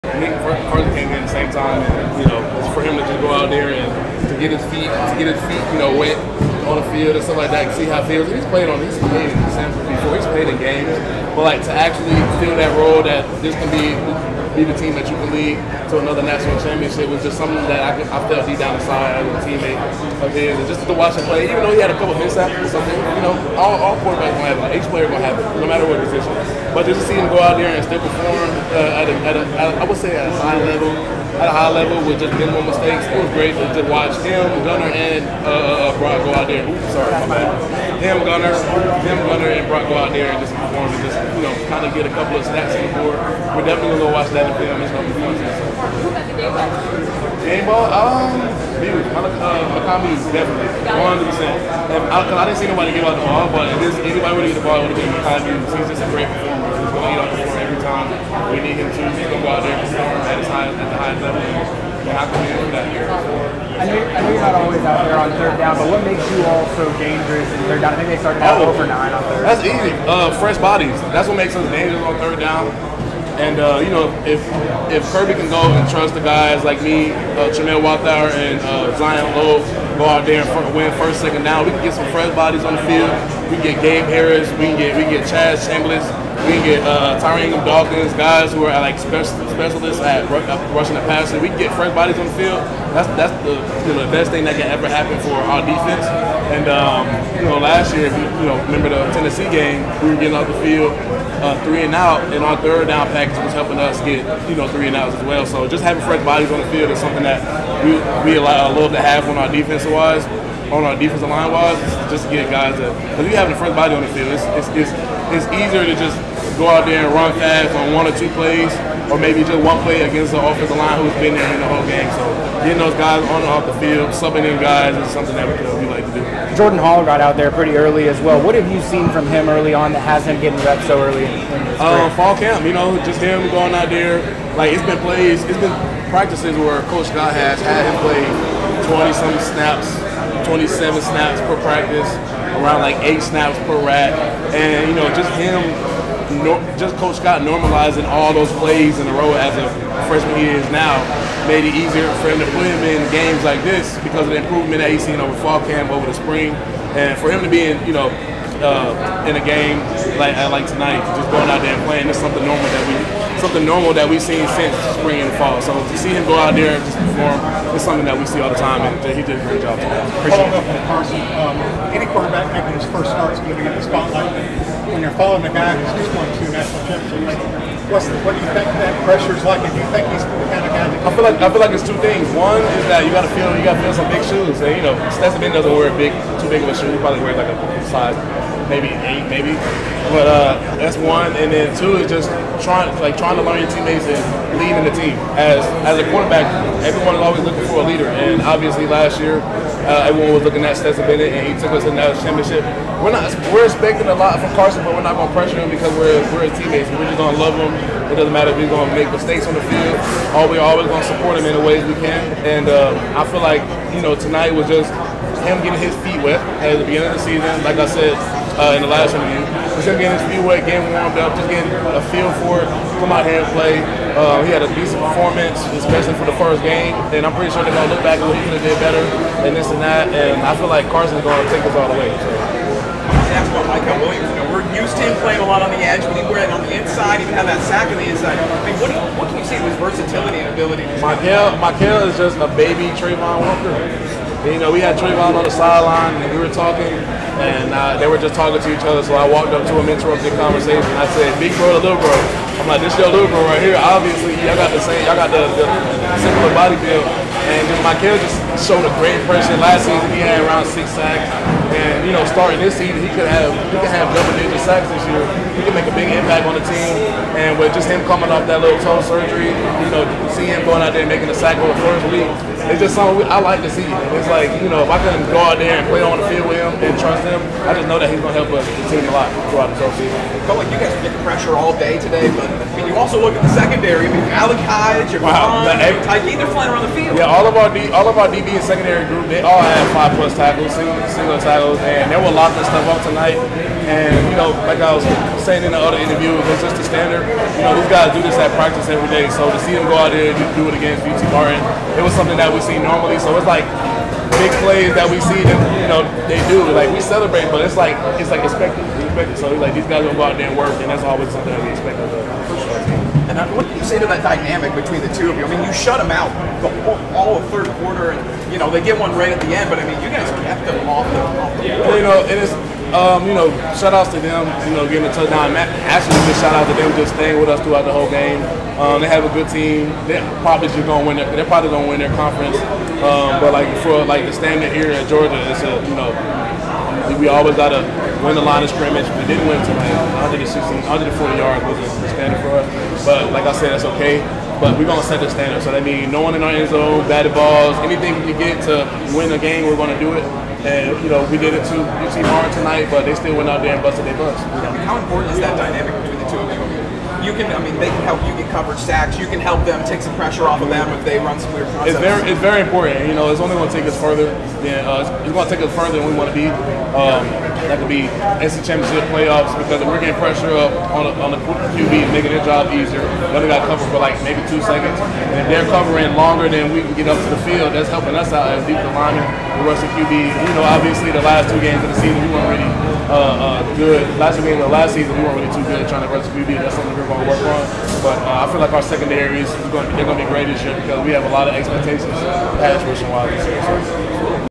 Nazir Stackhouse on Carson Beck and the secondary's performance
Here is the latest Bulldogs football video from Athens, Georgia, following UGA's 48-7 win over UT Martin.

You can find Kirby Smart press conference and UGA highlights at UGASports.com.